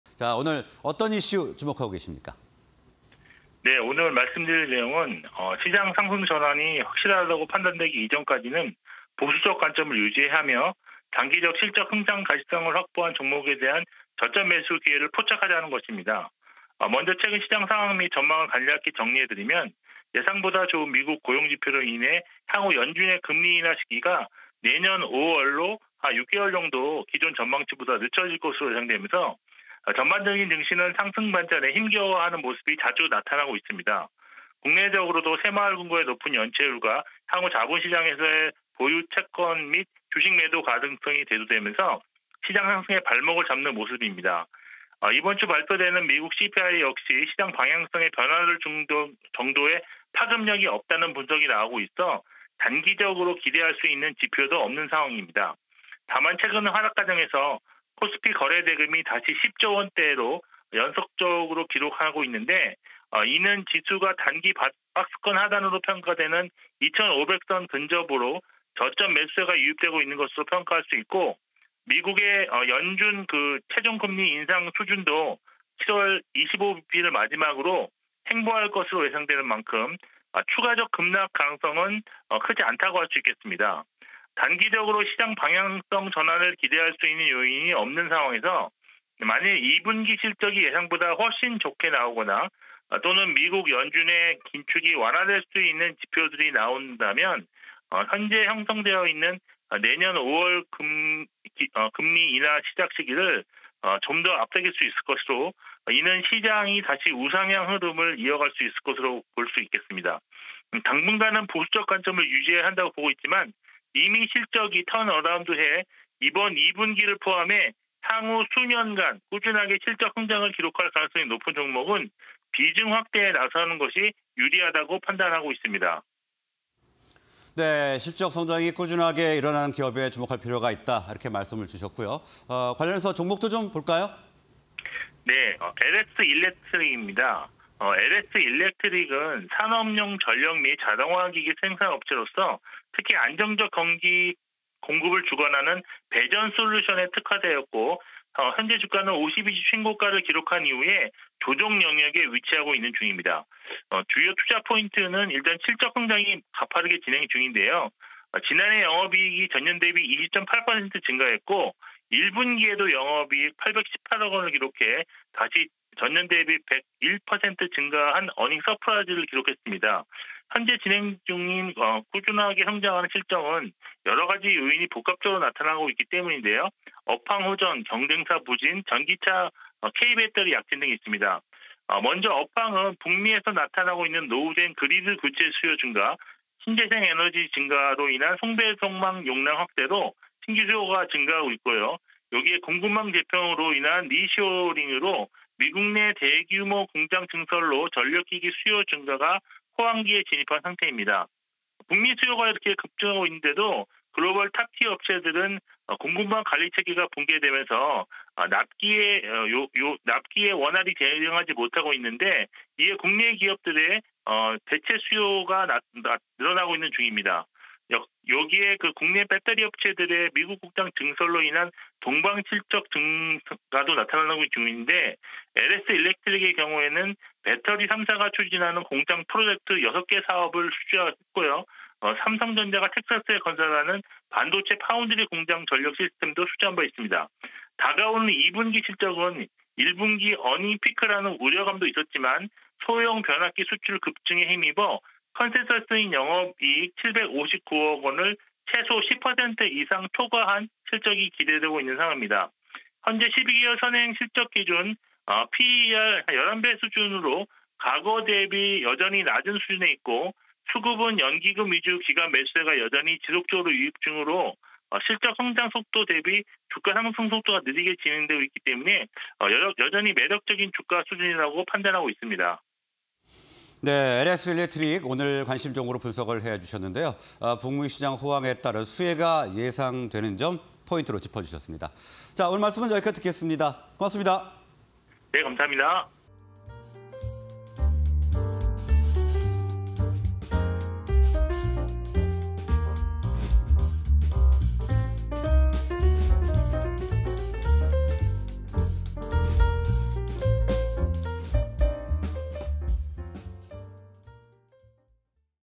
LS ELECTRIC(010120)... 7월 '숨고르기'… "실적 개선주는 달린다"?_투자 인사이트 (20230711)
Q. 7월 '숨고르기'… "실적 개선주는 달린다"?

LS ELECTRIC(010120)
- 북미 전력기기 업황 호조 수혜
- 글로벌 경쟁사 생산차질 따른 반사익
- K-배터리와 동반한 실적 성장 중
- 2분기 영업이익 컨센서스 초과 예상